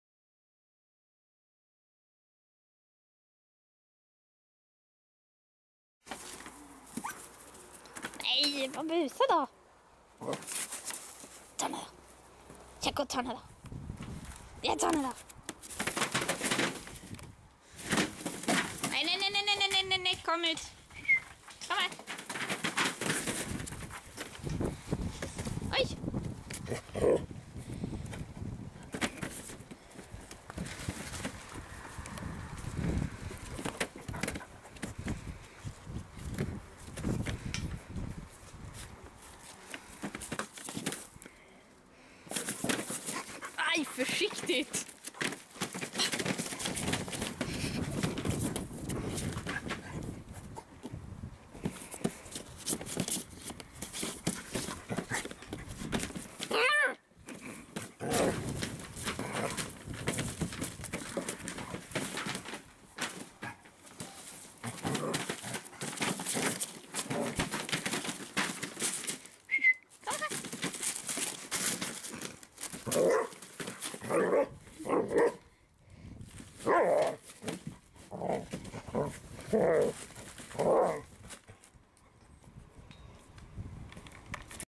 Busigt! 10/7
Holly, 3år, och Yessi, 9veckor, busar av sig på altanen!